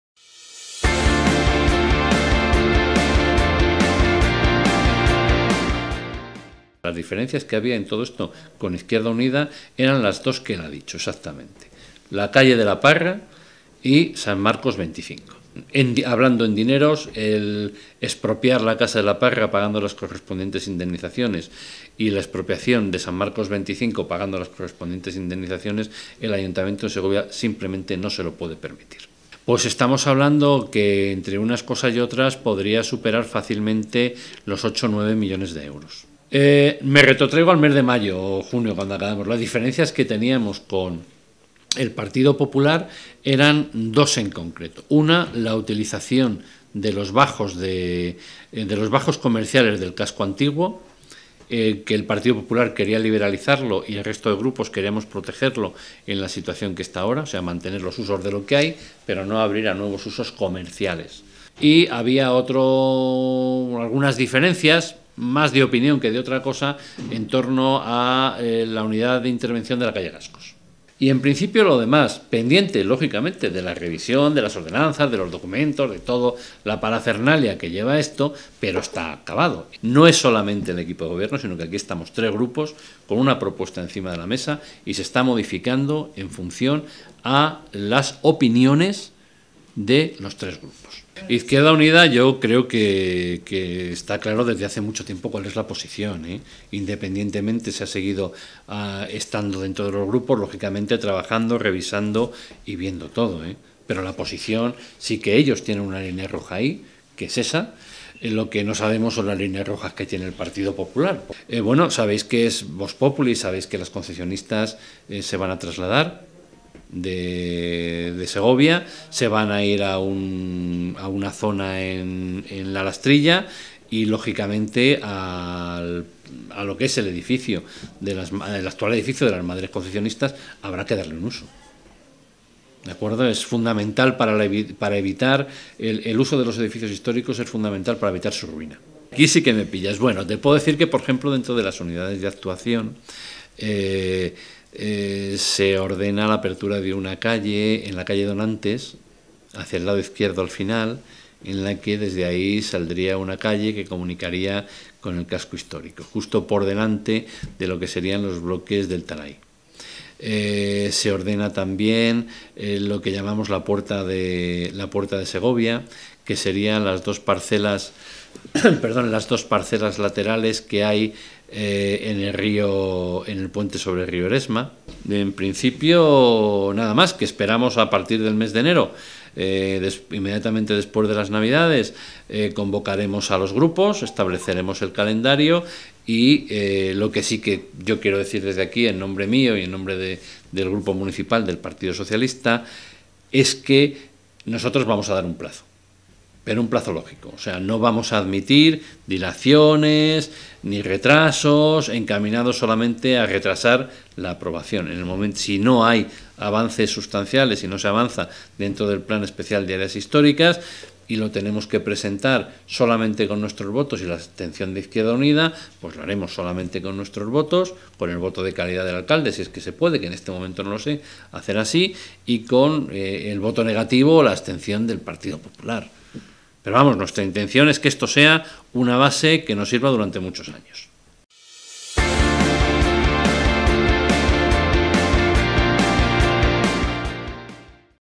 Alfonso Reguera, sobre el PEAHIS: "Vamos a dar un plazo lógico"
El alcalde, Pedro Arahuetes, ha dado tiempo a la oposición para intentar sacar adelante el Plan Especial de Áreas Históricas (PEAHIS) con el mayor consenso posible. Pero ese plazo no será ilimitado, tal y como ha declarado el concejal de Urbanismo, Alfonso Reguera, a 'Zoquejo.com'. "Vamos a dar un plazo lógico. No vamos a admitir dilaciones encaminadas solamente a retrasar la aprobación. Si no se avanza dentro del PEAHIS y lo tenemos que presentar solamente con nuestros votos y la abstención de IU,  lo haremos". En el presente vídeo, Reguera explica en qué momento se encuentra el Plan, cuáles son las diferencias entre los distintos grupos, cuál es el calendario previsto y avanza algunas de sus características.